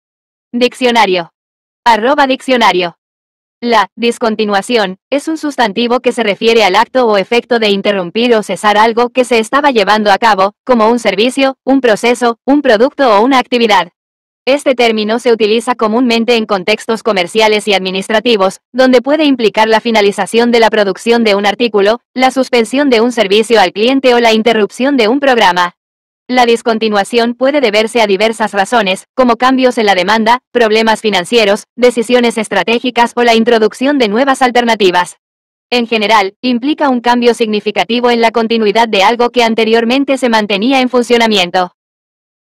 Discontinuación Significado En Español
La "discontinuación" es un sustantivo que se refiere al acto o efecto de interrumpir o cesar algo que se estaba llevando a cabo, como un servicio, un proceso, un producto o una actividad. Este término se utiliza comúnmente en contextos comerciales y administrativos, donde puede implicar la finalización de la producción de un artículo, la suspensión de un servicio al cliente o la interrupción de un programa. La discontinuación puede deberse a diversas razones, como cambios en la demanda, problemas financieros, decisiones estratégicas o la introducción de nuevas alternativas. En general, implica un cambio significativo en la continuidad de algo que anteriormente se mantenía en funcionamiento.